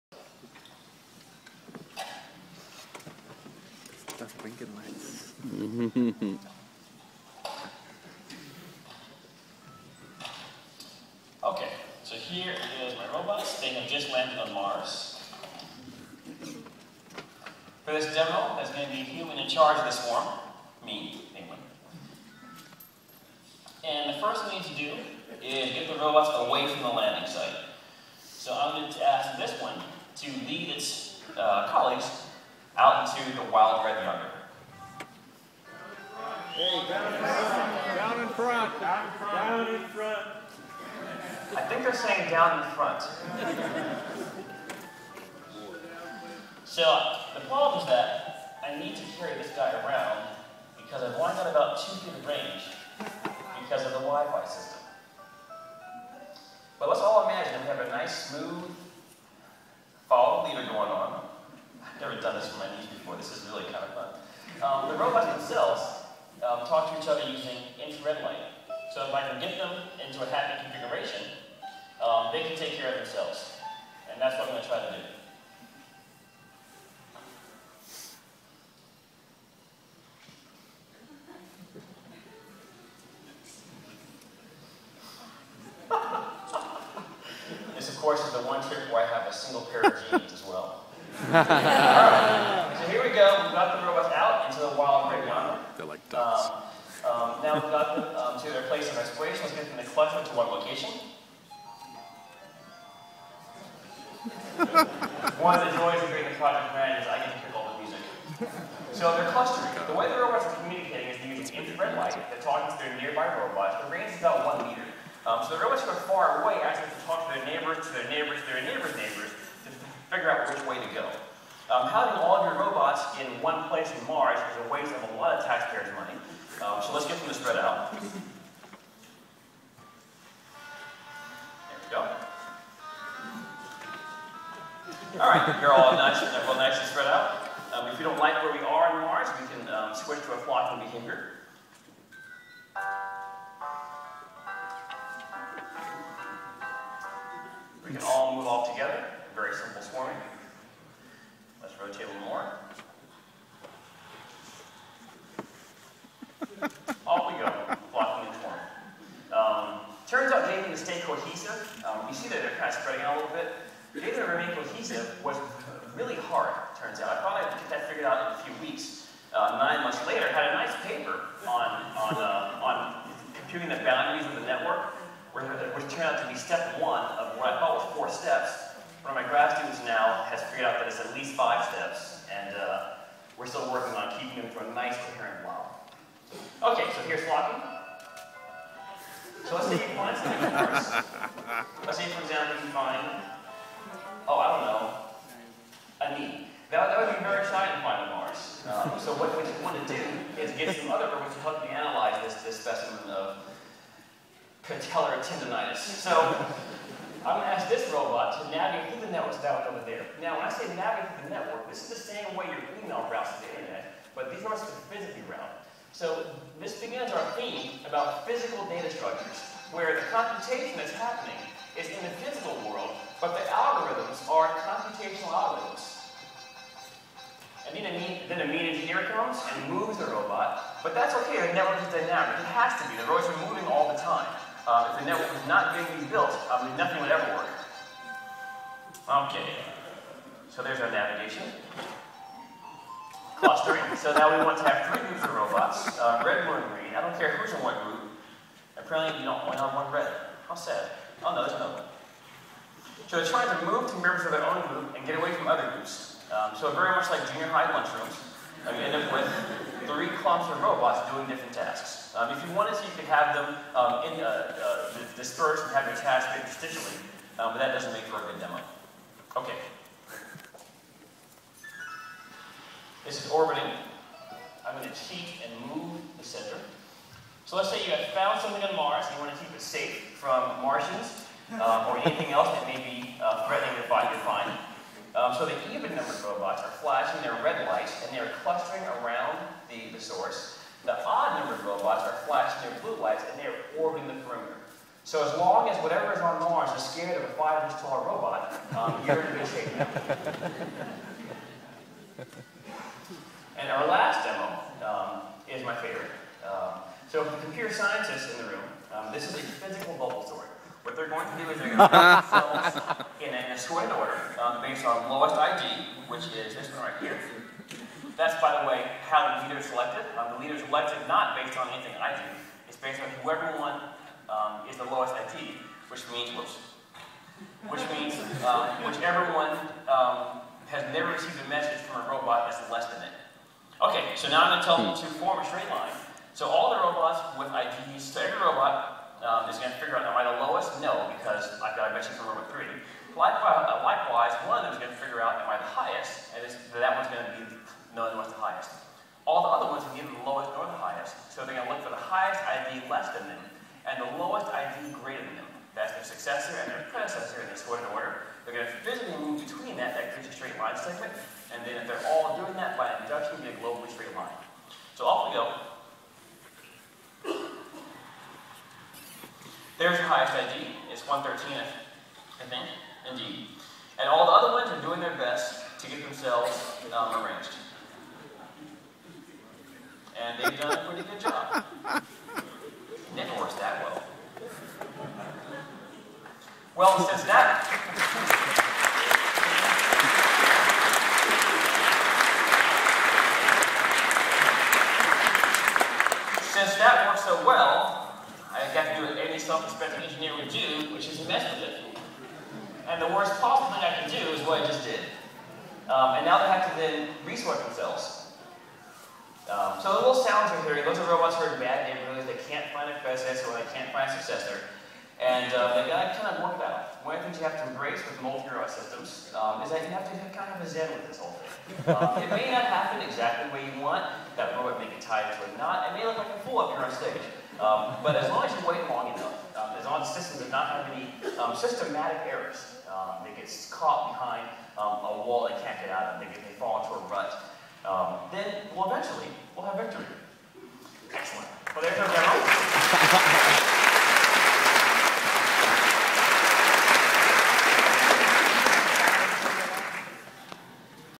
Professor McLurkin's Fabulous Robots
Demo of swarming robots at Singularity Summit 2011